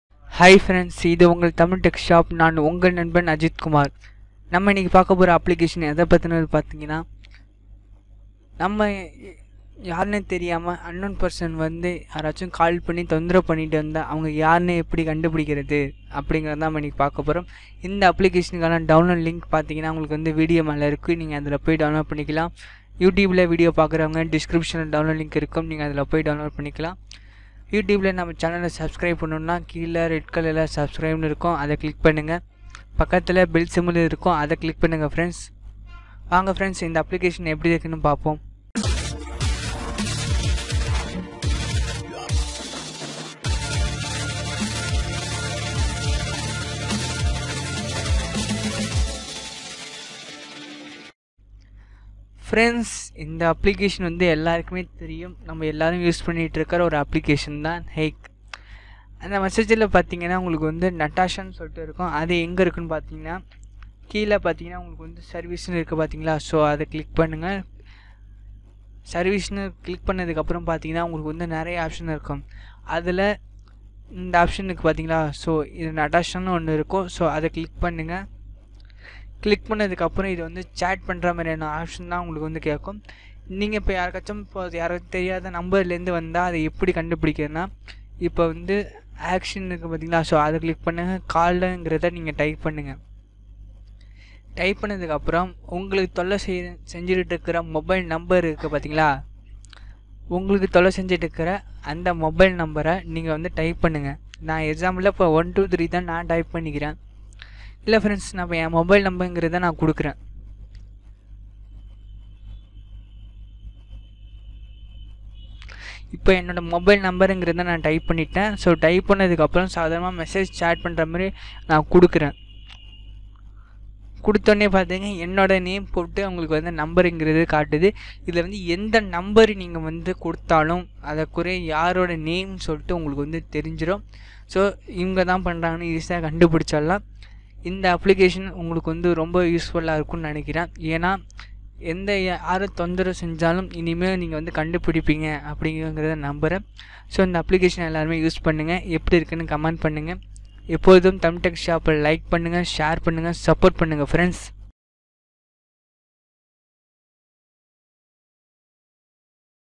How To Trace Unwanted Calls? Trace Phone Number (Tamil)
அன்பு உறவுகளே
உங்களுக்கு யாரன்று தெரியாமல் call வந்து தொல்லை செய்பவர்களின் பெயர் தெரிய வேண்டுமா? வீடியோ காணுங்கள்.